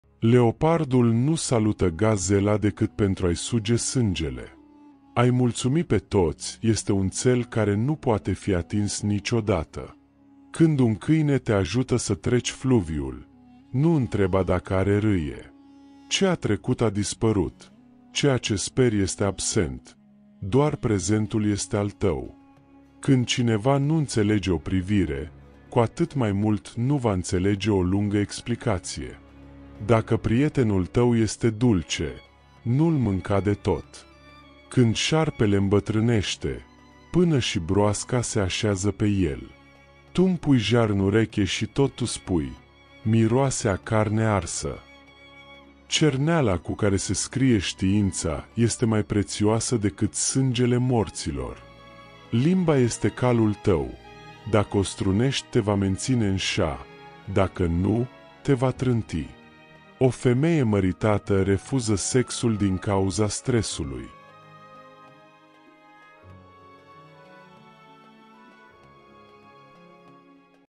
Partea a 4-a: Proverbe Arăbești despre Viață - Înțelepciune din Tradițiile Arabe
Te invităm să urmărești ultima parte din seria noastră dedicată proverbelor arăbești! În această selecție, vei descoperi noi zicători care subliniază valorile esențiale ale vieții, curajul și înțelepciunea poporului arab. Aceste proverbe sunt transmise din generație în generație, oferind sfaturi prețioase pentru orice situație de viață.
 Ce vei găsi în Partea a 4-a:
Proverbe arăbești care îți vor schimba perspectiva
Zicători arabe despre iubire, muncă și destin
Tradiții și înțelepciune arabă pentru a înfrunta provocările